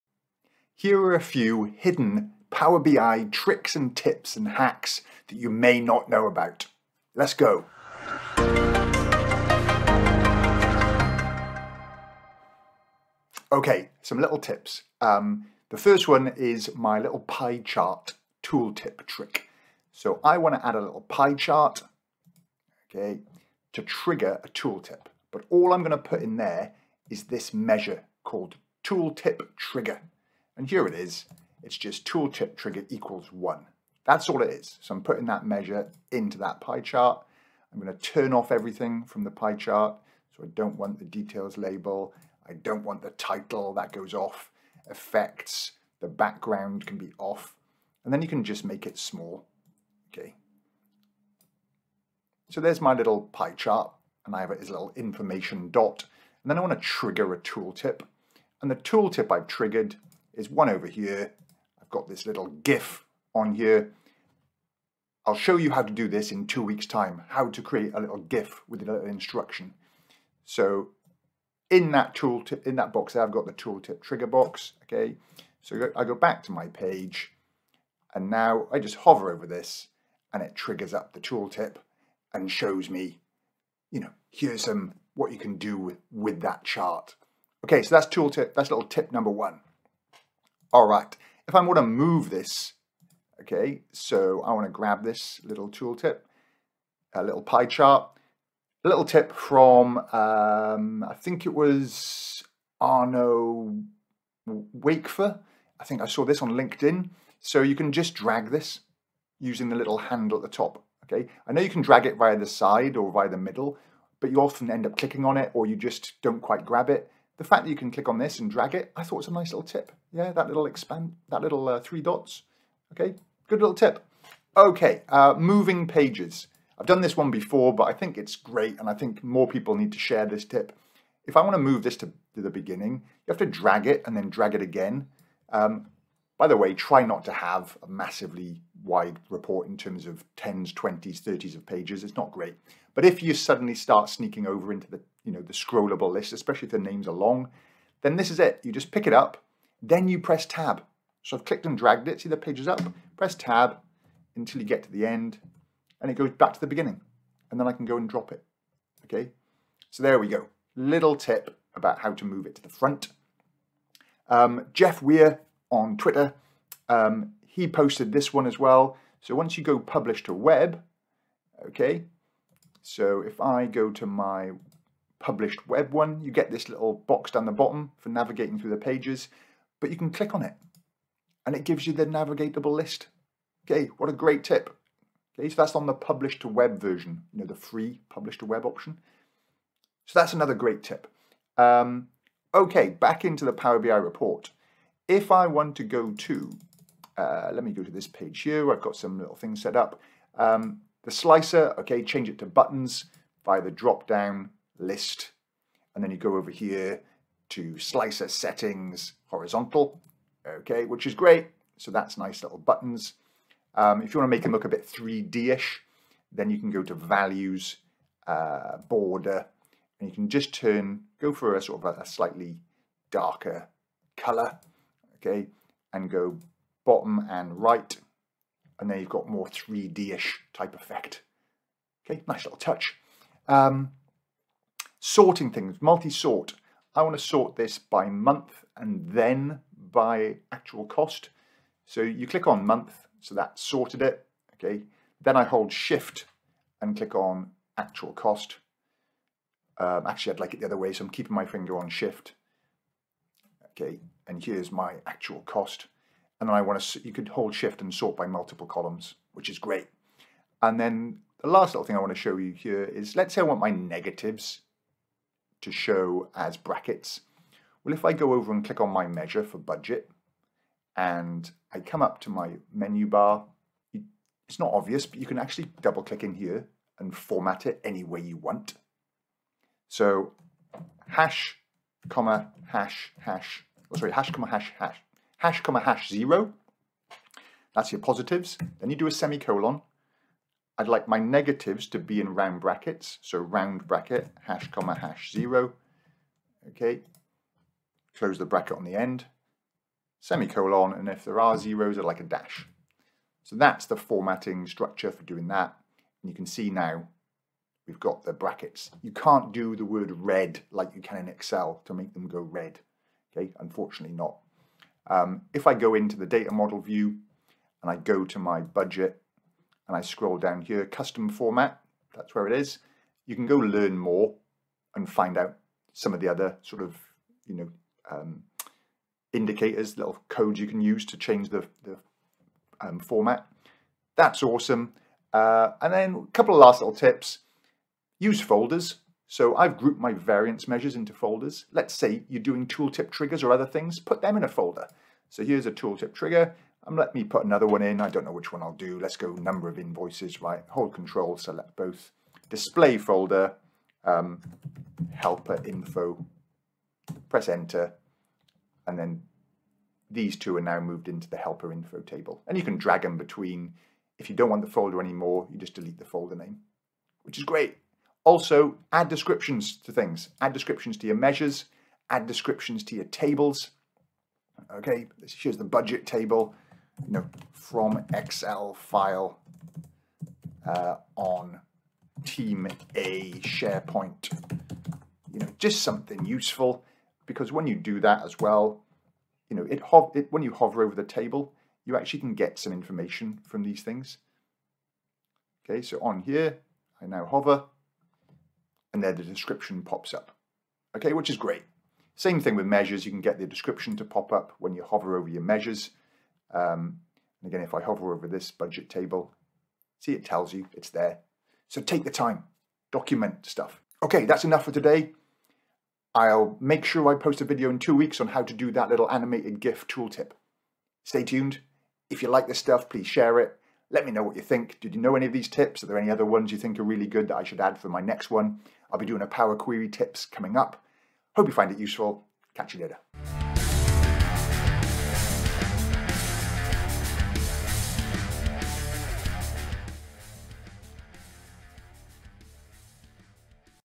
Power BI Hidden Tricks and useful hacks
How many of these Power BI tricks and features did you know about?

00:00 Intro
00:20 The Pie Chart Tooltip trigger
01:40 Grab the 3 dots to drag visuals
02:00 Drag and Tab to move page to the beginning
02:45 Page navigation in Publish to Web experience
03:13 Slicer to buttons with 3D effect
03:56 Sort Multiple Columns holding SHIFT
04:24 Formatting negatives with (brackets)
05:28 Putting measures (or columns) in folders
06:10 Add descriptions to measures and tables